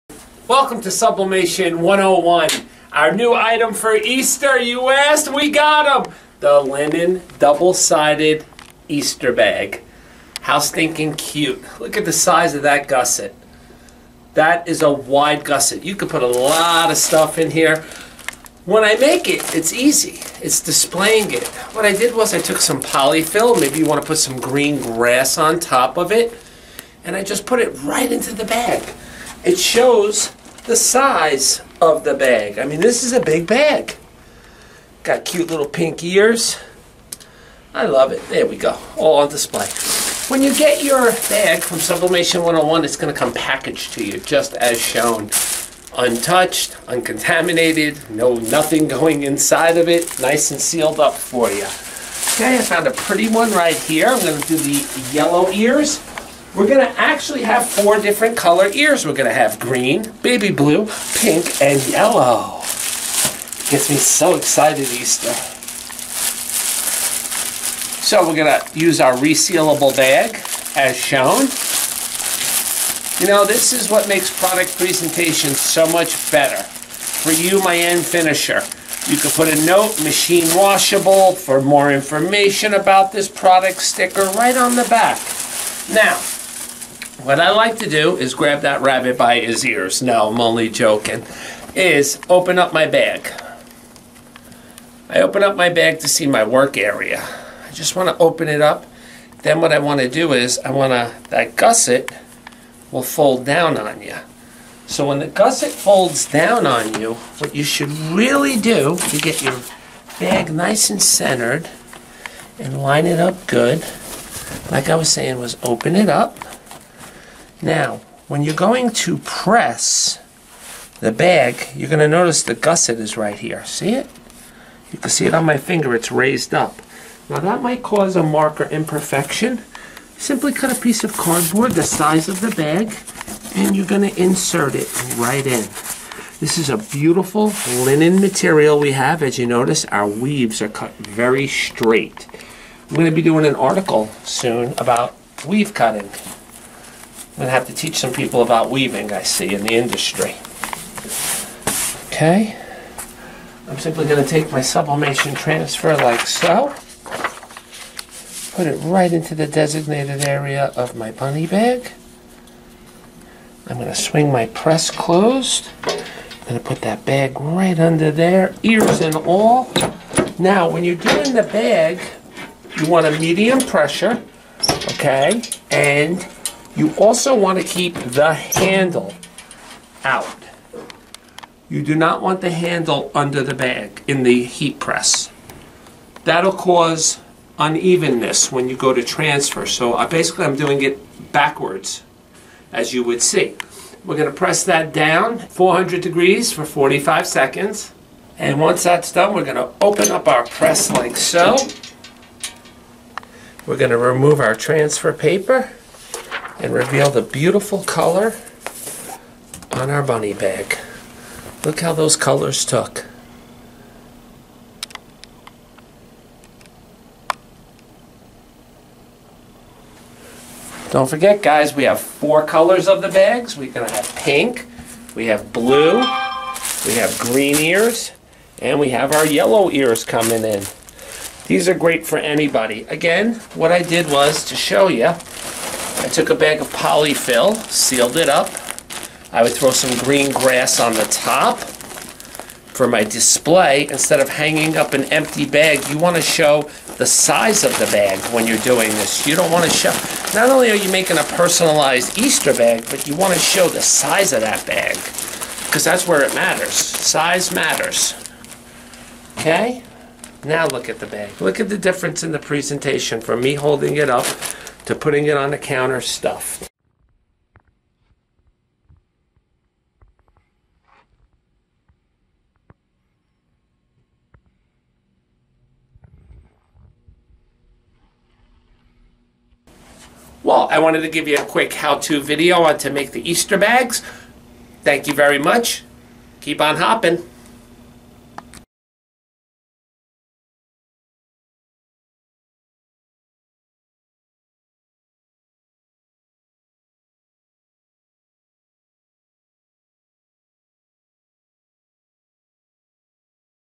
Sublimation101 NEW Item Easter Bunny Ears Bag!
These are our latest bags!   Coming SOON!
Directions:
400F 45S
Using a cardboard insert to help flatten the bag (especially the gusset) is advised.

@Sublimation101 endeavors to make the process of sublimation quick, easy and profitable.  We test what works and what doesn't so that you don't have to!